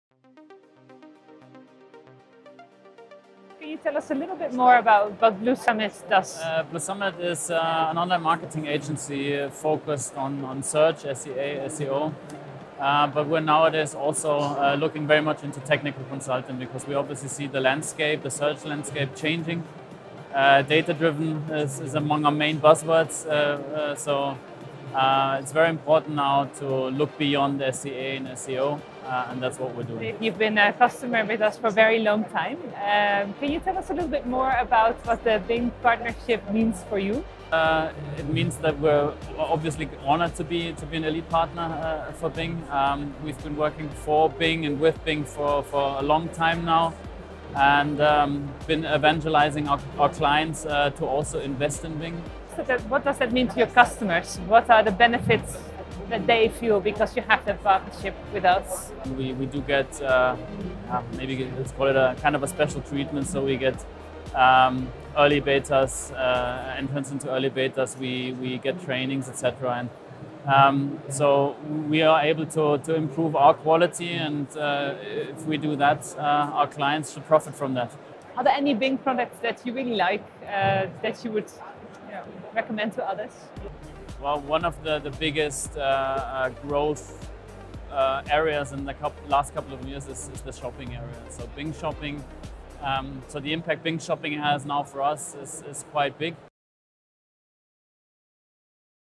Interview with Blue Summit on the value of being a Bing Partner
We had the chance to interview Mirko Teichert from Blue Summit on the value of being a Bing Partner.

Want to learn more about the Bing Partner program?